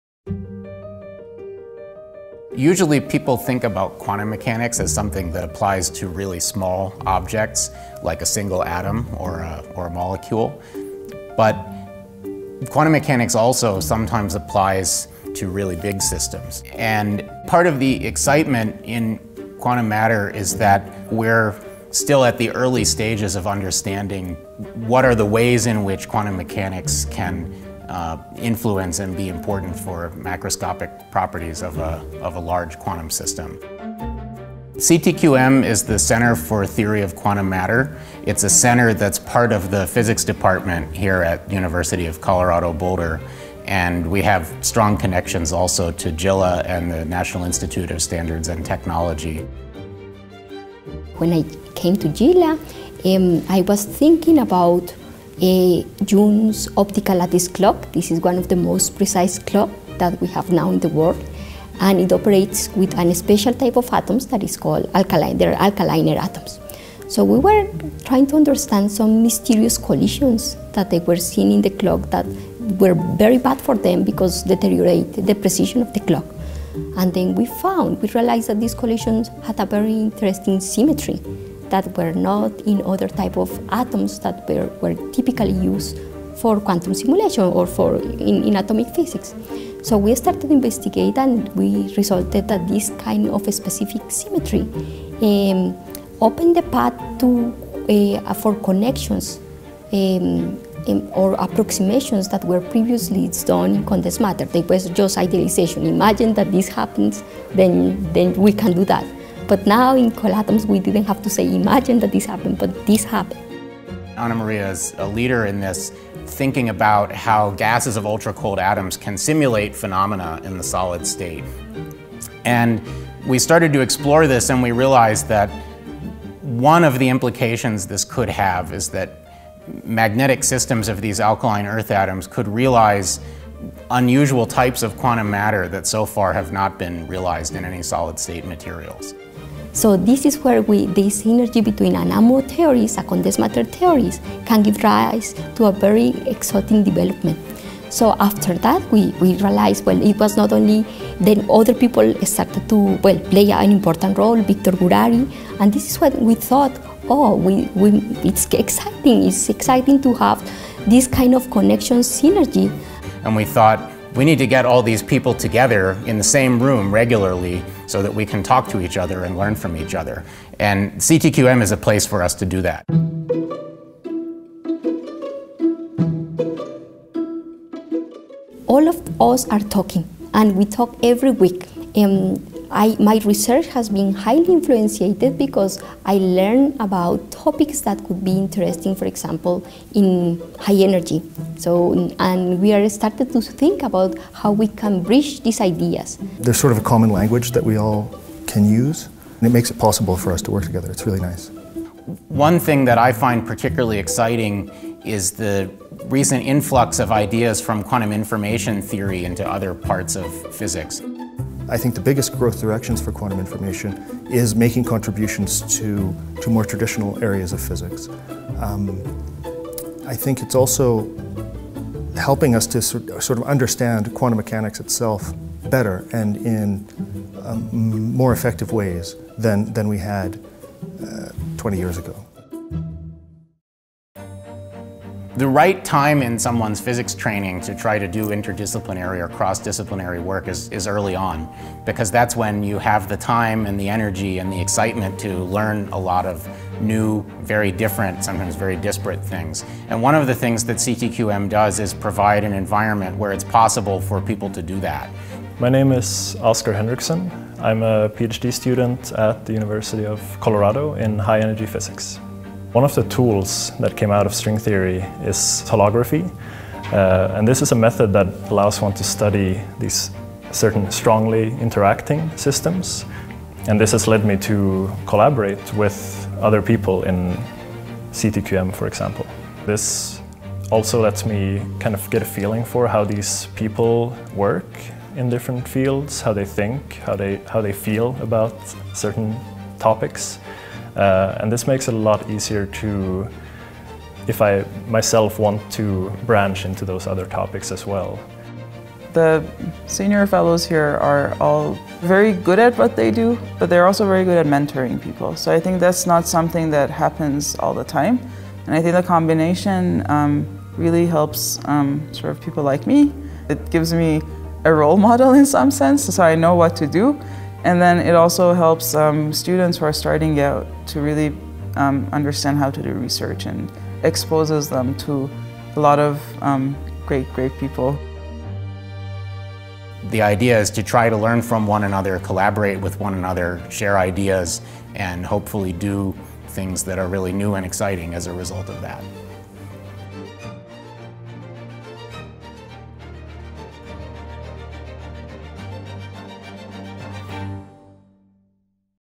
Center for Theory of Quantum Matter: Connecting Through A Common Language
The Center for Theory of Quantum Matter (CTQM) is a newly created center within the Physics Department at the University of Colorado Boulder (CU). CTQM was founded in 2014 and conducts theoretical physics research focused on macroscopic quantum matter, ranging from electrons in solids to quark-gluon plasmas, to trapped ultra-cold atoms, to systems engineered to process quantum information, and even to black holes.
The topic of quantum matter is a focal one that transcends traditional discipline boundaries, and CTQM brings together theoretical physicists working in five different sub-fields (atomic, molecular and optical (AMO) physics; condensed matter physics; high-energy physics; nuclear physics; and quantum information science). While Center members come from different sub-fields, work at CU, JILA or the National Institute of Standards and Technology (NIST), and have different backgrounds, the similarities among the problems they study are more fundamental than the differences. The Center is a vibrant, dynamic and welcoming community where students, postdocs and faculty carry out research at the frontiers of theoretical quantum matter. Center members exchange ideas and collaborate across traditional discipline boundaries to advance the frontiers of our understanding of quantum matter.